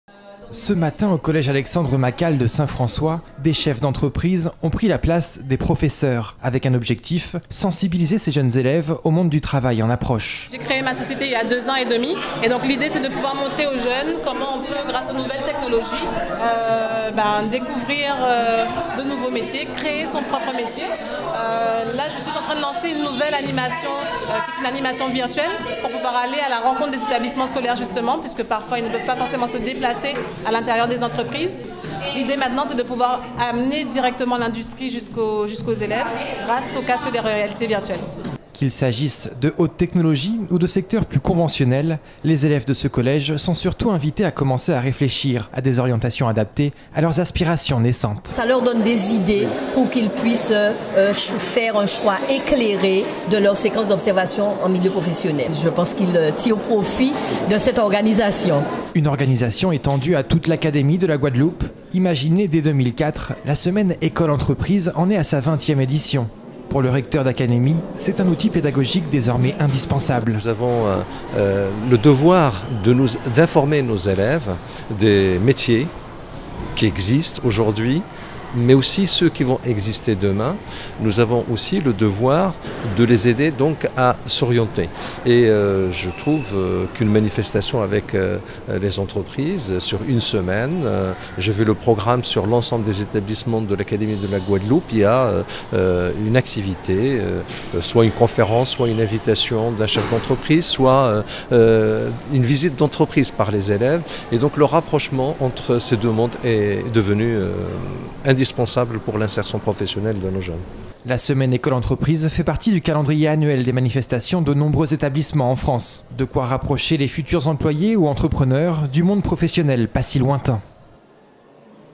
🔴 Des chefs d'entreprises dans les écoles pour initier les élèves au monde du travail !
"Le rapprochement entre ces deux mondes est désormais indispensable!"

La 20ème semaine école entreprise démarrait ce matin au collège Alexandre MACAL de Saint-François ⤵️